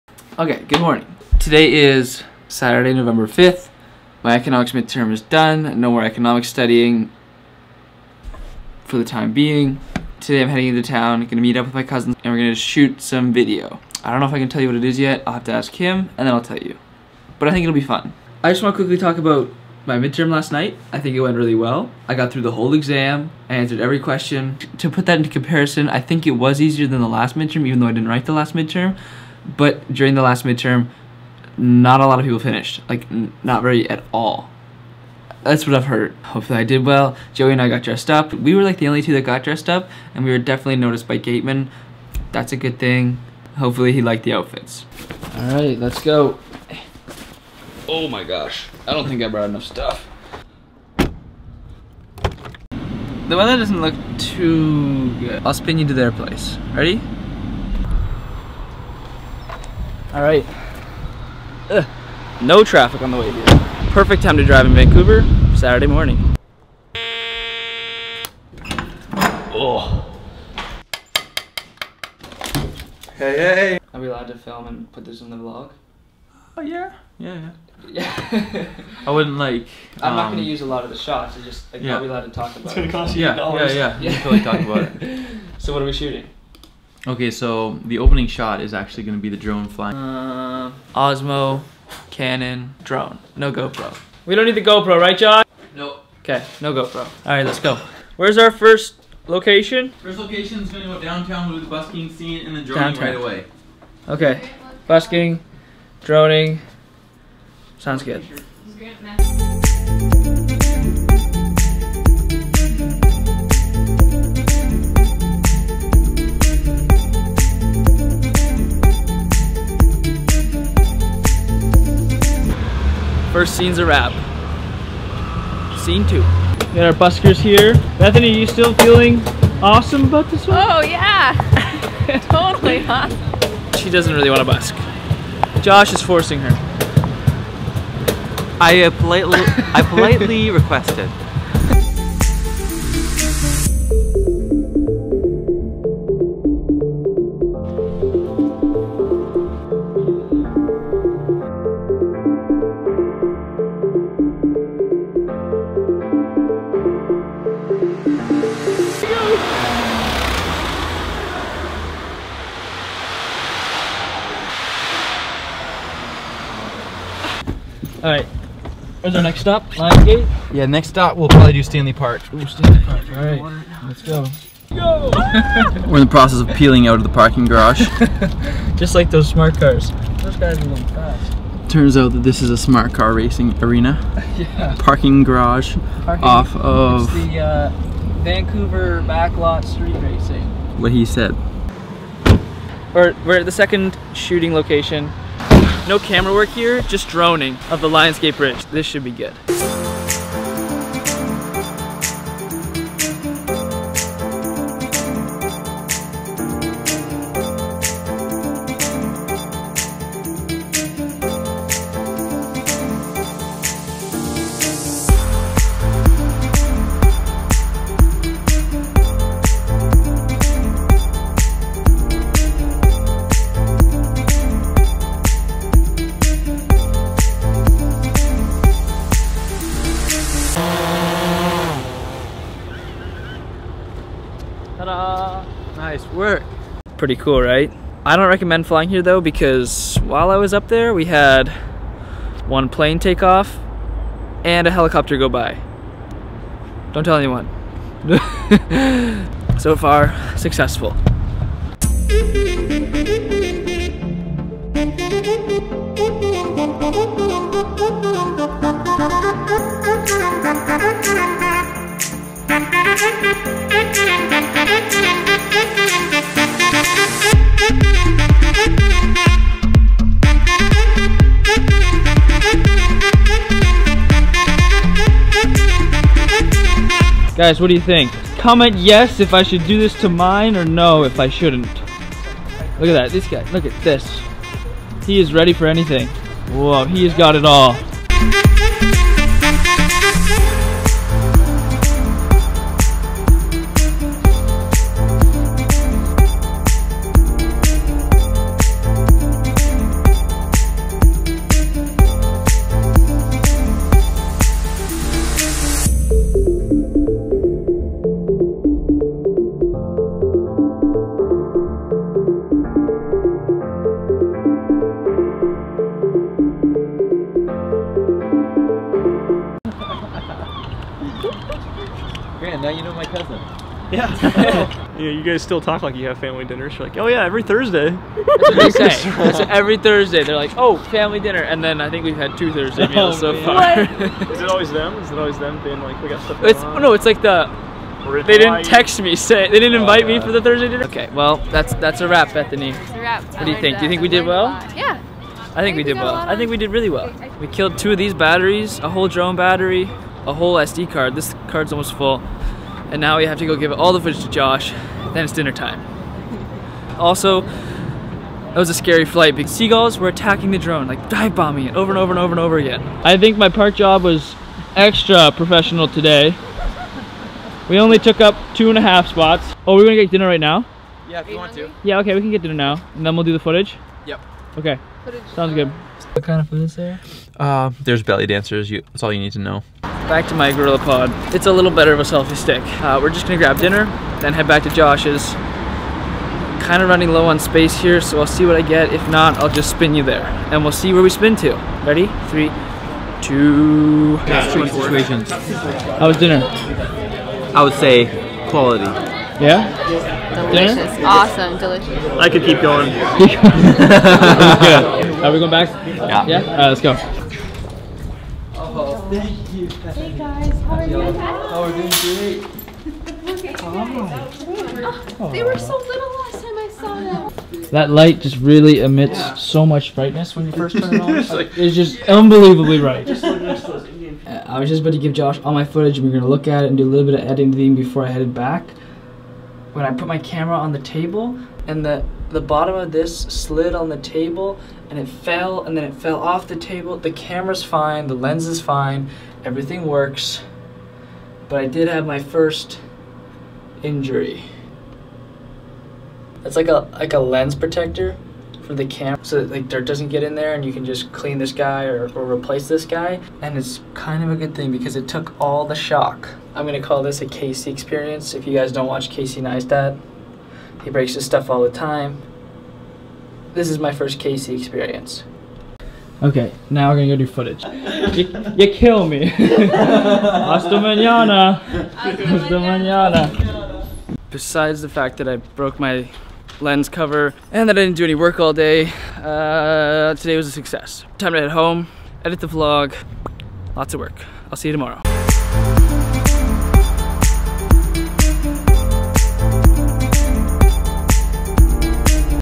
MAKING A MUSIC VIDEO!! VLOG 057
Got to spend the day with family!! So nice and now it's time to do a lot of essay work!!